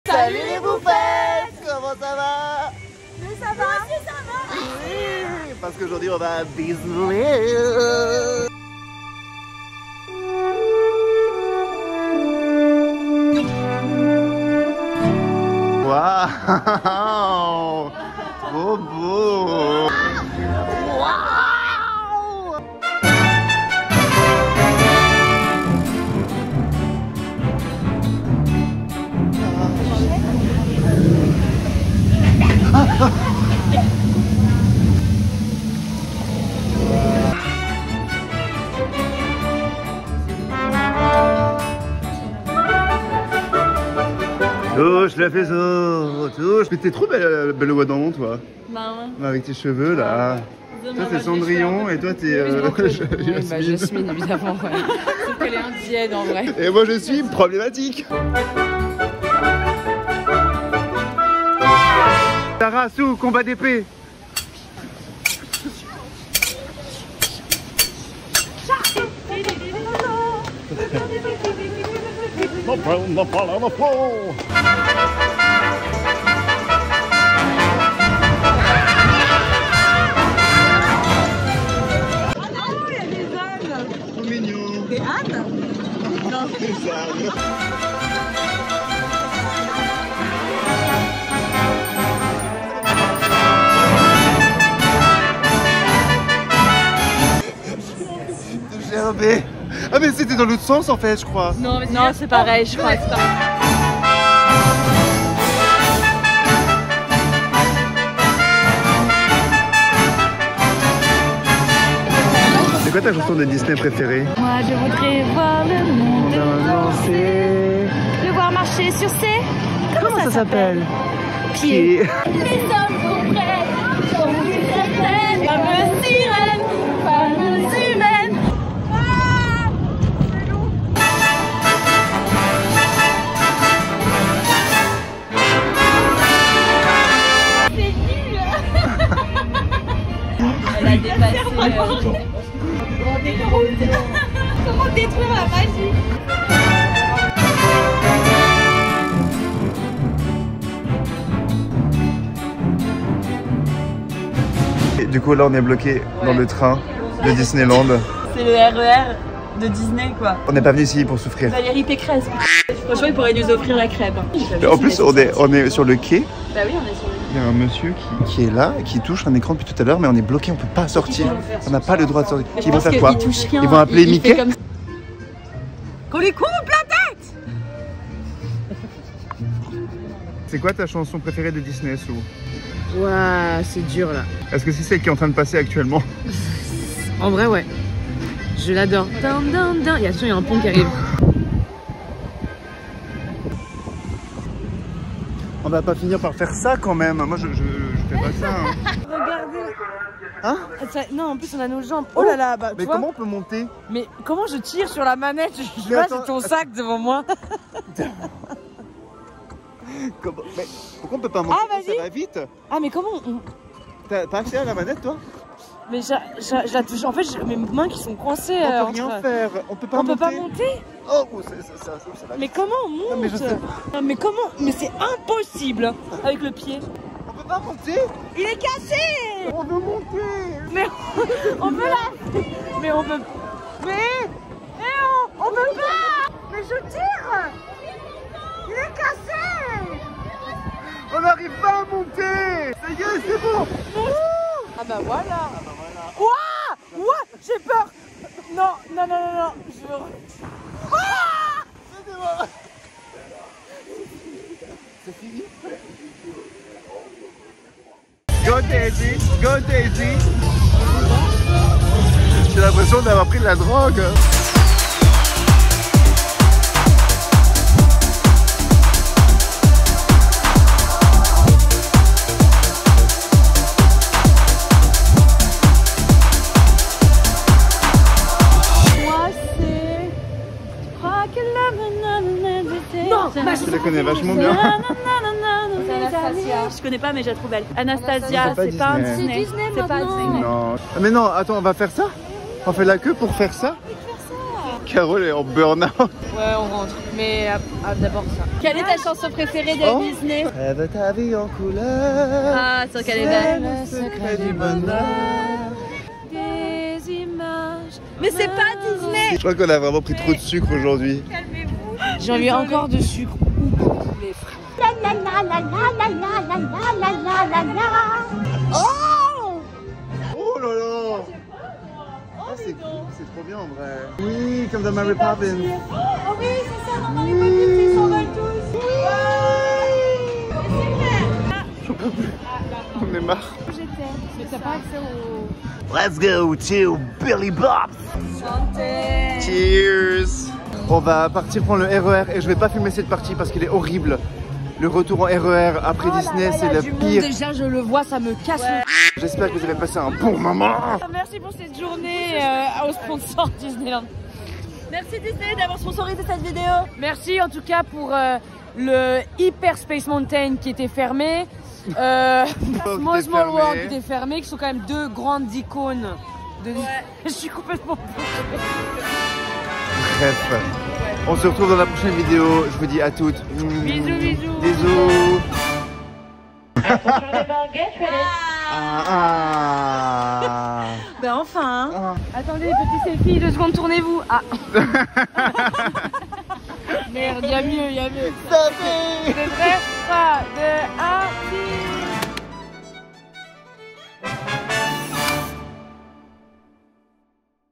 EN ROUE LIBRE À DISNEYLAND notsponsored (ft. Sara, Caro et Sou)
J'en ai encore un poing de côté.

À toute
- Mouffy